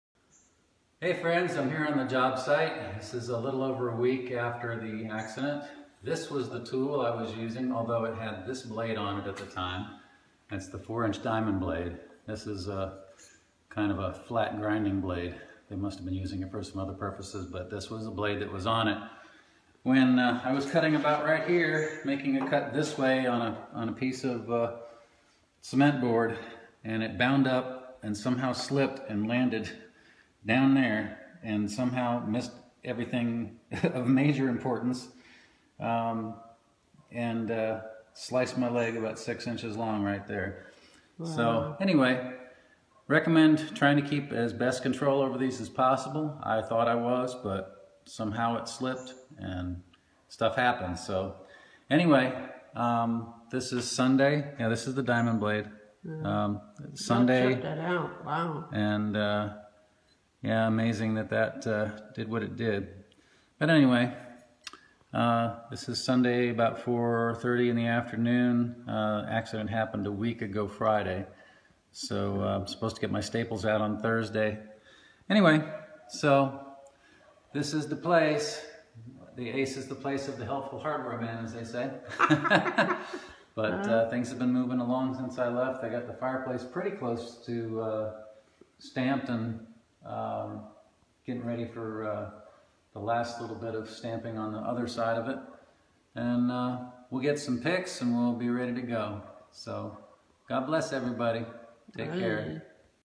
Near death accident with 4 inch diamond blade grinder
While at work on Friday morning, March 23 2018, I lost control of a 4 inch diamond blade grinder, which proceeded to fall into my pants, cutting about a 6 inch long by 1 inch deep gash in my upper left thigh, totally exposing my sapheneous vein (the largest vein in our legs) 360 degrees around, yet miraculously did not cut it! I may upload another video presenting the actual wound, but for now, suffice it to say that the doctors were saw amazed, they began taking  pictures of the wound and sharing it with their collegues all over the hospital! The day before the accident occured, the Lord had spoken to me through a dream, saying, "I will restore all of what you have lost", but also, through a facebook friend, spoke the word, "I will turn your scars into stars"!  On of my doctors also told me that some time ago...he had to do a proceedure to expose a saphenous vein, but even with his skill and much sharper and more precise surgical tools, he actually nicked the vein in the process. Praise God  for His hand of protection...and that I am still alive! This happend one week before Passover...and this first born son is thankful I'm under the blood of the spotless lamb of God, our Messiah, Yeshua (Jesus)!!